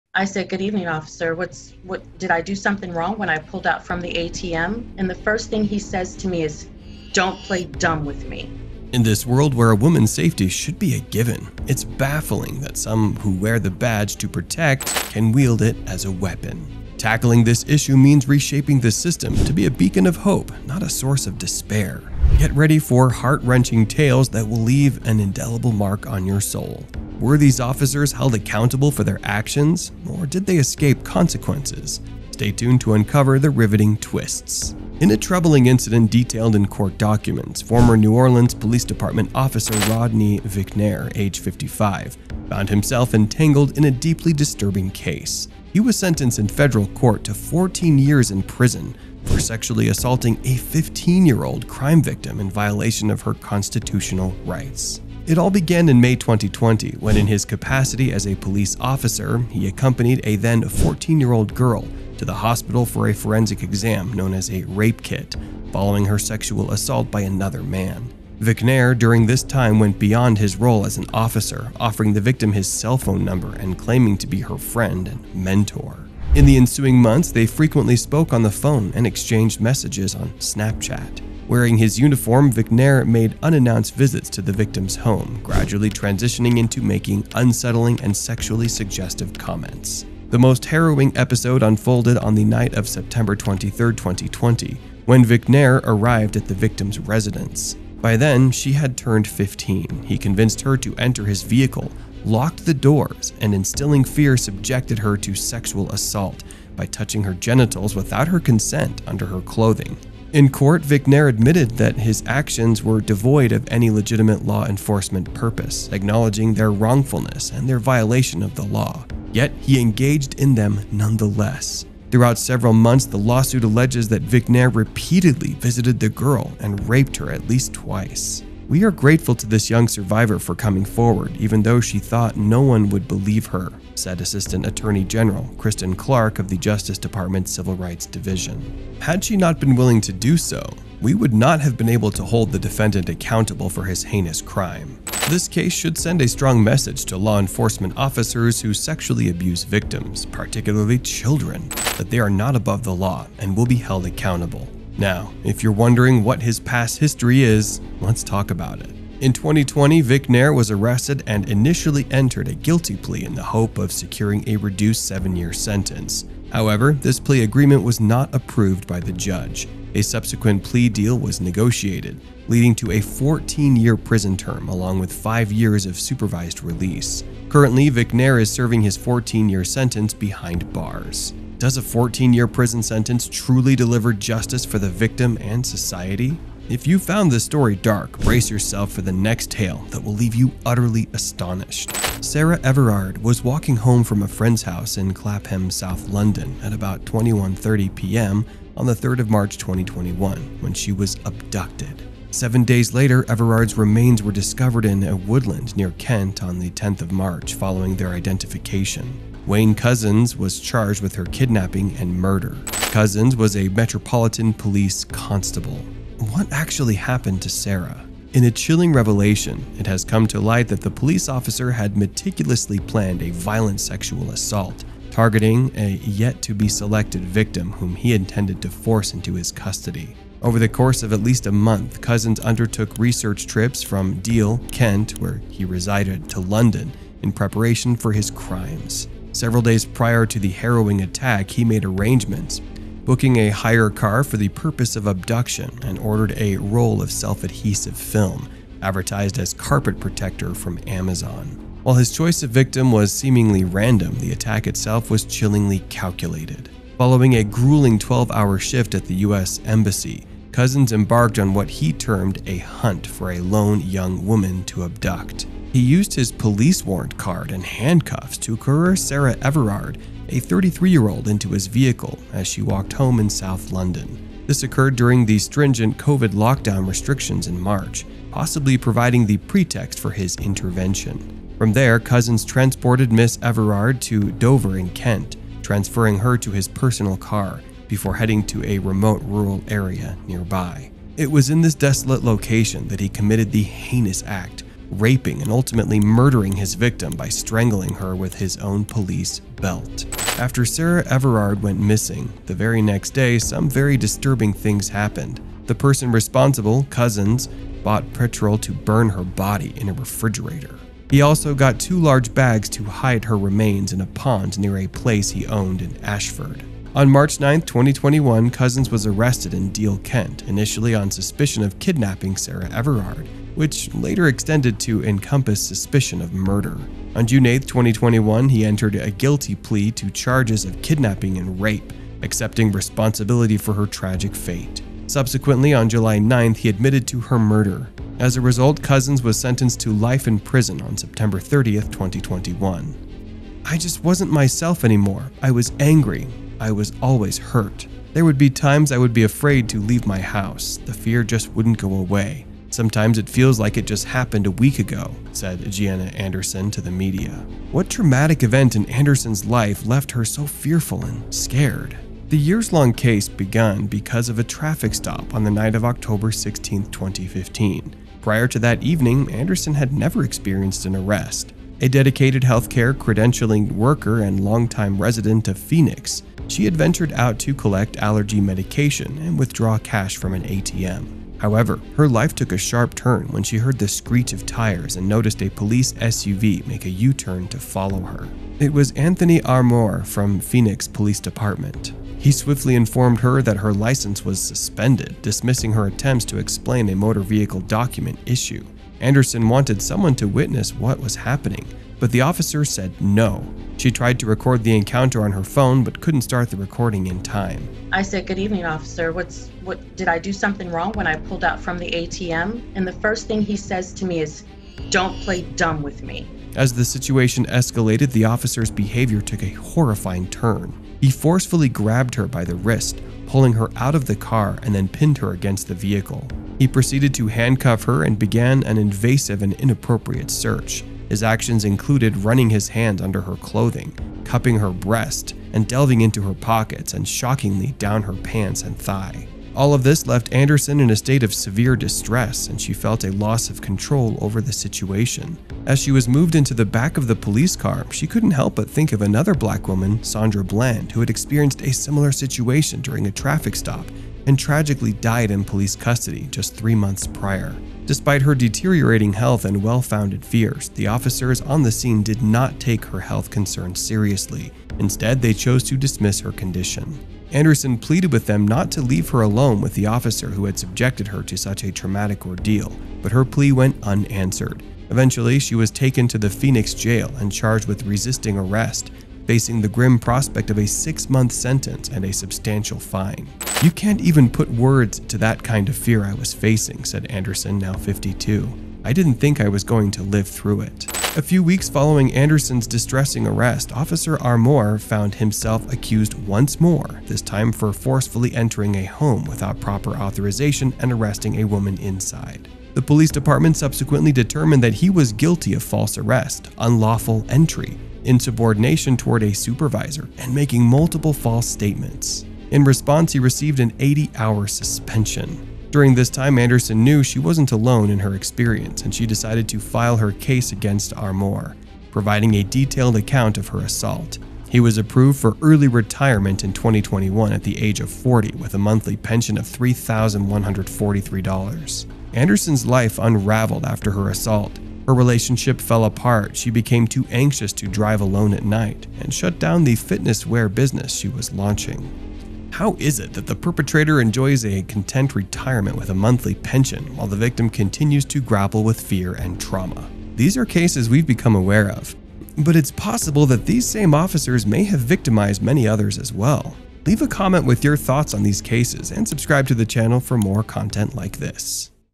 Cops Harass Girl, What Happened Next Is SHOCKING!
In this world, where women's safety should be a given, it's baffling that some who wear the badge to protect can wield it as a weapon. Tackling this issue means reshaping the system to be a beacon of hope, not a source of despair.

Get ready for heart-wrenching tales that will leave an indelible mark on your soul.

Were these officers held accountable for their actions, or did they escape consequences?